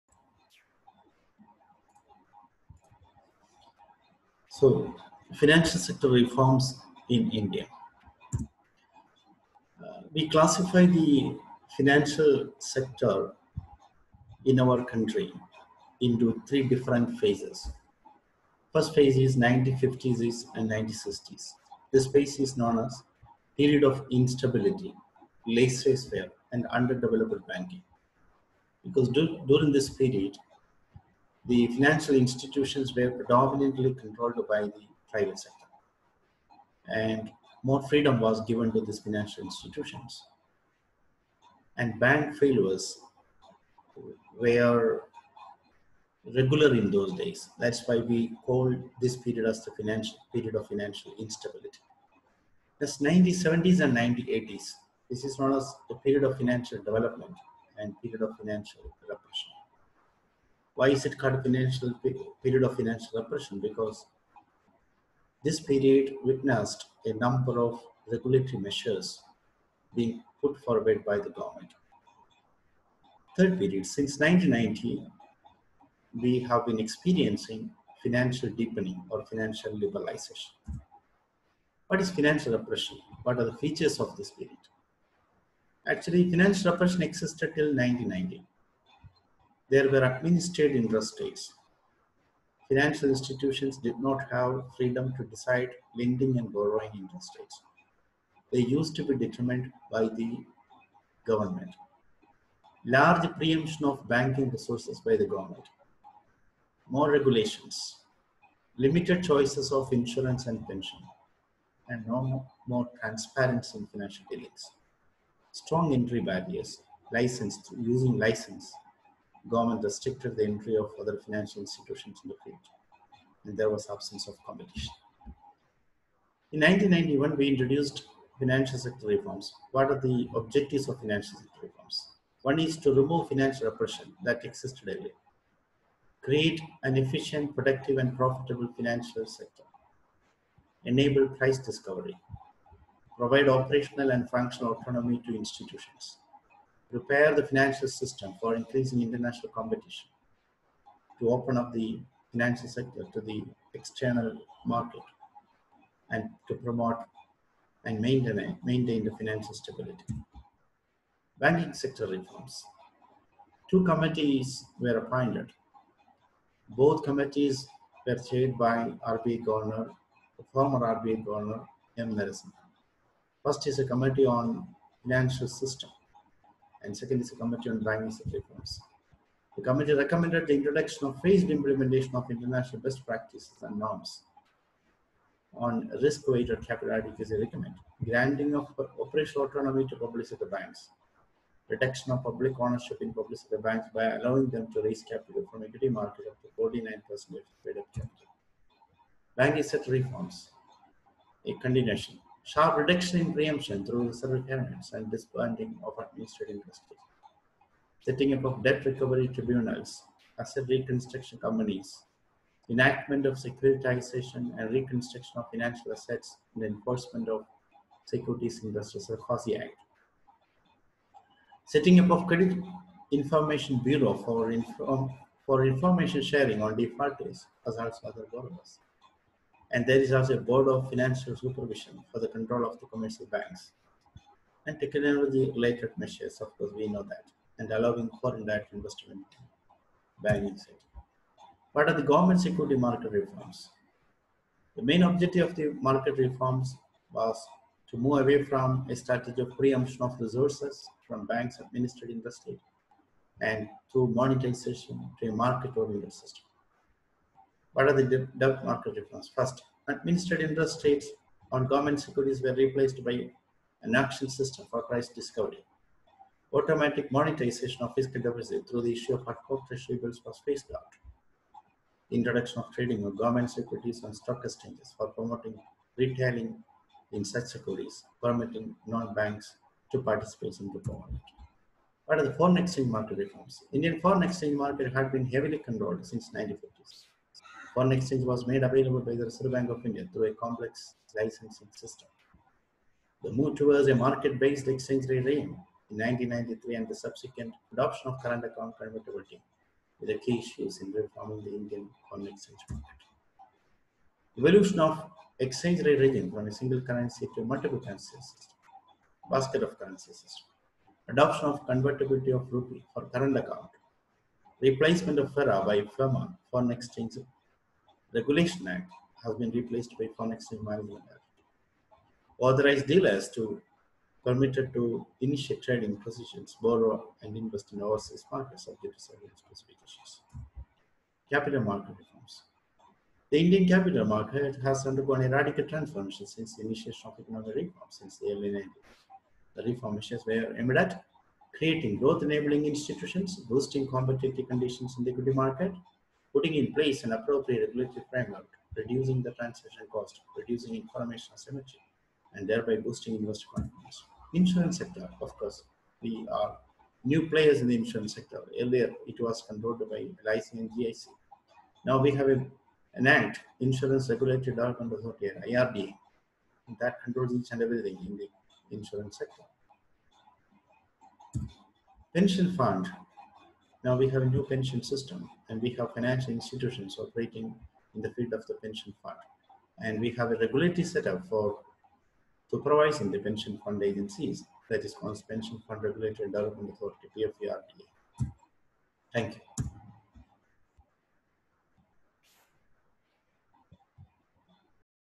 Financial Sector Reforms
This video discusses important aspects of financial sector reforms in India. Students of Economics and those who prepare for competitive exams like UGC/NET, PSC, UPSC may find this video useful.